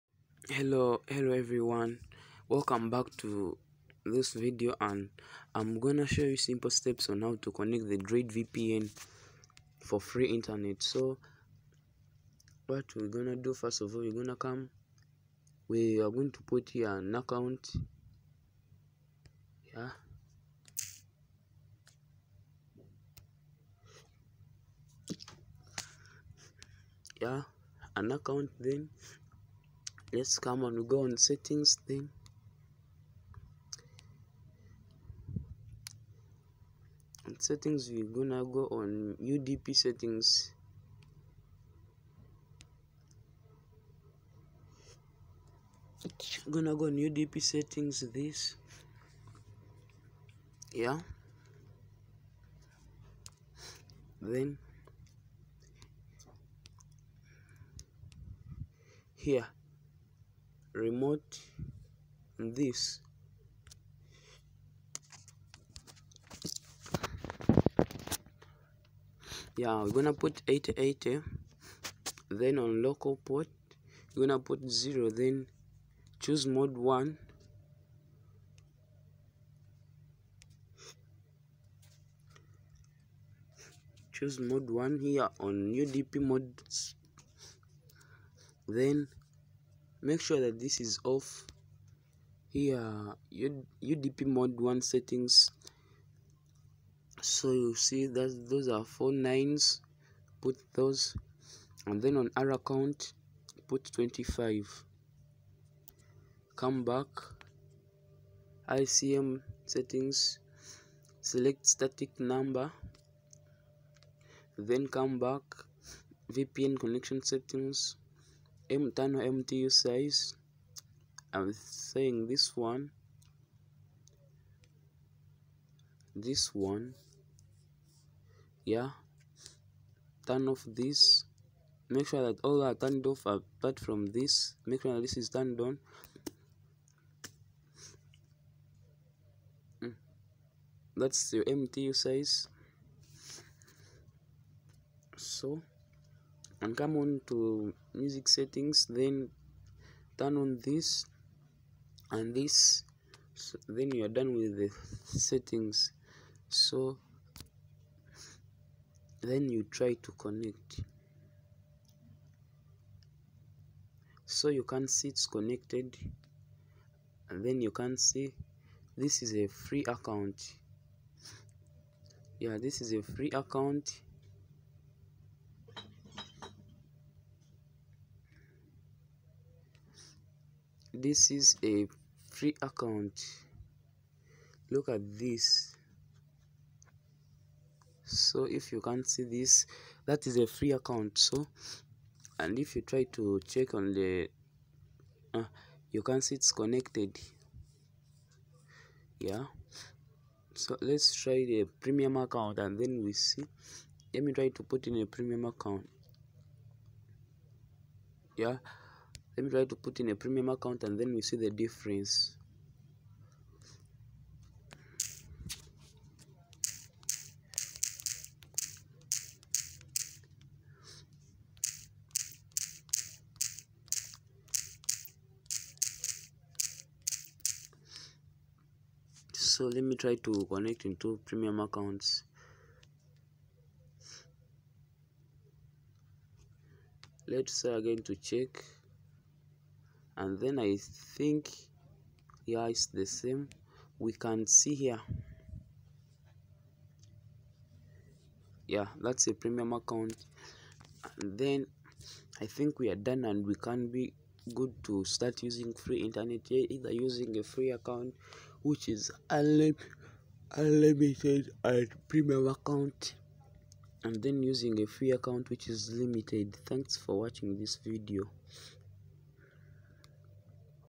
Droid Vpn premium settings 2021
After them blocking the hosts that were used in My country Uganda... another hope is here ..Bye Ha tunnel..now let's use Droid Vpn